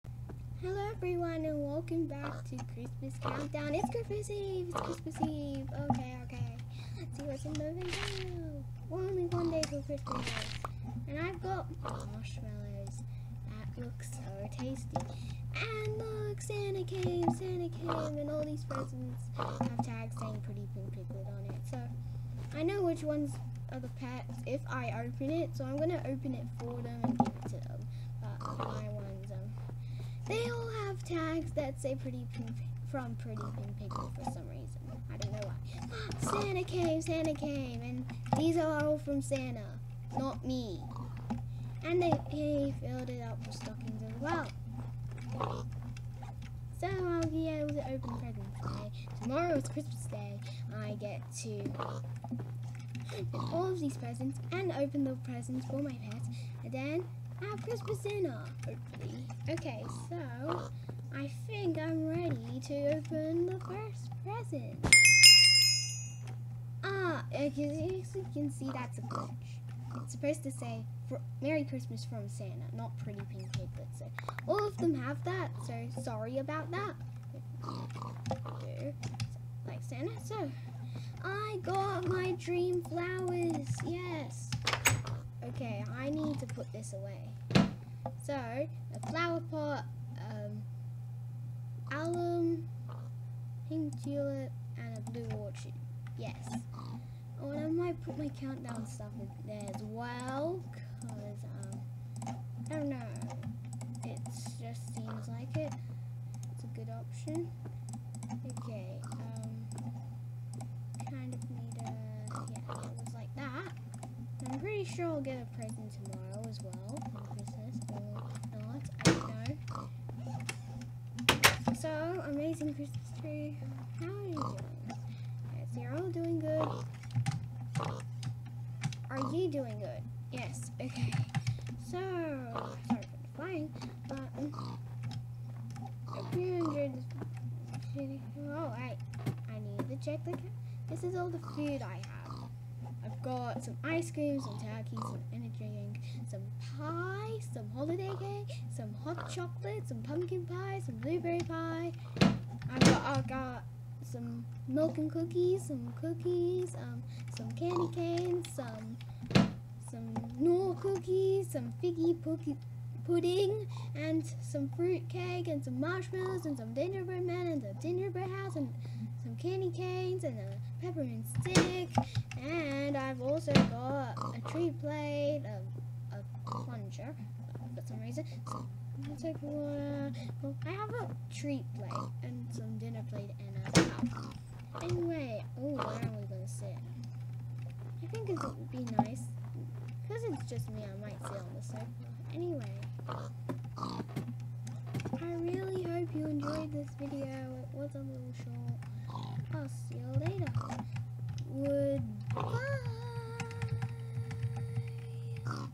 Christmas eve - Day 5
Today we open are very first Christmas present.